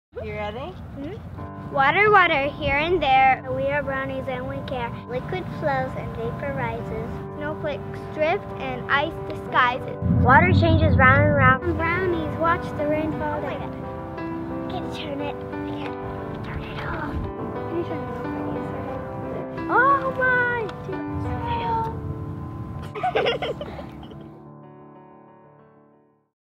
"Water Water" Girl Scout Rhyme
Brownie's of Troop 677 recite a rhyme as part of their "Wonders of Water" journey with help from a Junior working on her "Agents of Change" journey.

By Blue-Skye Spence with Ft. Myers Beach Girl Scout Troop 677.